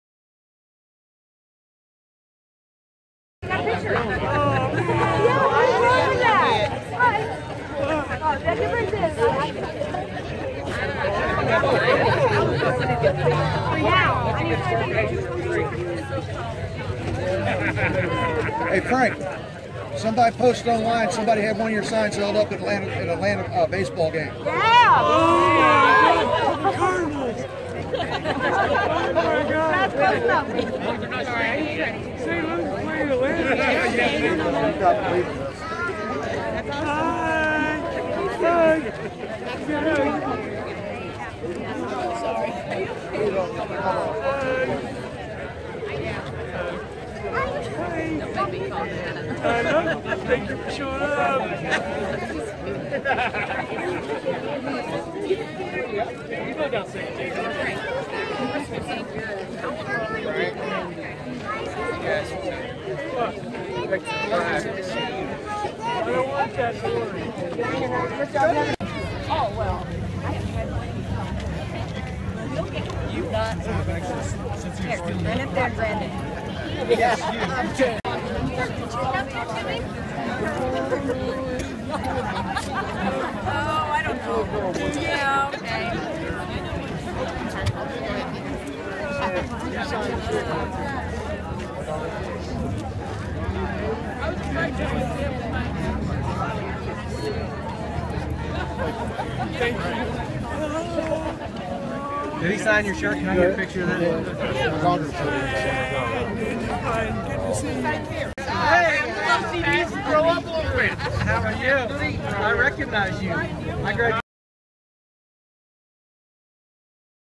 Ozark Radio News - Hugs for Frank event
Hundreds gathered in the Southern Hills Shopping Center Saturday, April 9 for the "Hugs for Frank" event. Frank Swanson was fired from the West Plains Walmart April 2. Walmart says that Swanson allowed items to leave the store without being paid for, while Swanson and his family say termination papers he was given indicated he was fired for price matching and for hugging customers.